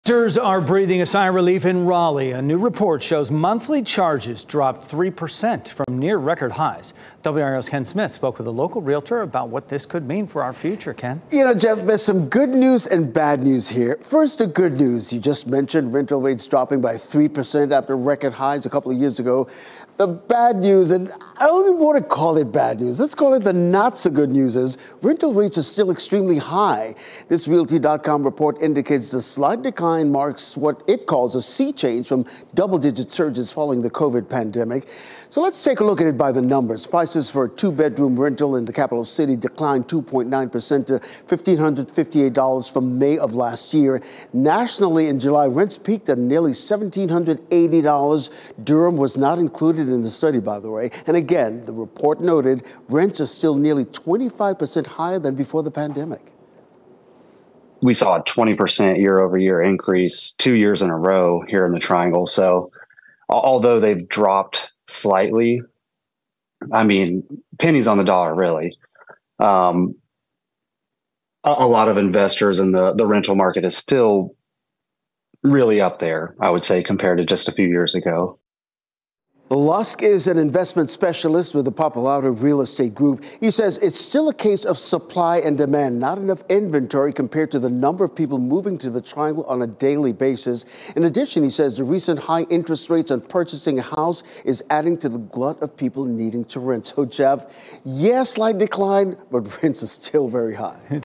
Rents fall nearly 3% in Raleigh; 2 Bedroom was $1,694 (July '22) now avg. $1,558 (June '23)
Renters are breathing a sigh of relief in Raleigh. A new report shows monthly charges dropped 3% from near-record highs. WRAL's Ken Smith speaks with a local realtor about what this could signal for the future. Renters in Raleigh are finally seeing some relief in prices with monthly charges declining almost 3% from near record highs, says a new report from Realtor.com. And lower rents are likely on the way.

The decline marks a “sea-change” from double-digit surges in the wake of the COVID pandemic, Realtors.com says. Prices for rentals up to two bedrooms in the capital city declined 2.9% to $1,558 from May 2022, when rents were approaching their post-2019 pandemic high, the realtors group said. (Durham is not included in the report.) Rents peaked nationally in July 2022 at nearly $1,780 with Raleigh rents at $1,694 – a 13.3% surge year over year according to Realtors.com data.

However, the report noted that rents are still nearly 25% higher than before the pandemic. Nationally, rents also were down but only 0.5% across the nation’s largest 50 metros to $1,739, based on Realtor.com data.

“In May, we saw the first year-over-year decline in rents, a sea-change from the double-digit growth that renters contended with in much of 2021 and 2022. This is yet another sign that rental-driven inflation is likely behind us, even though we may not see this trend in official measures until next year,” said Realtor.com Chief Economist Danielle Hale in a statement. “Although still modest, a decline in rents combined with cooling inflation and a still-strong job market is definitely welcome news for households.”

More declines may be coming, too.

“Looking forward, we expect to see a continued, albeit small, year-over-year decline in rental prices throughout the remainder of the year,” Hale said. Renters may find themselves with more bargaining power and may have better luck finding an affordable unit this year.”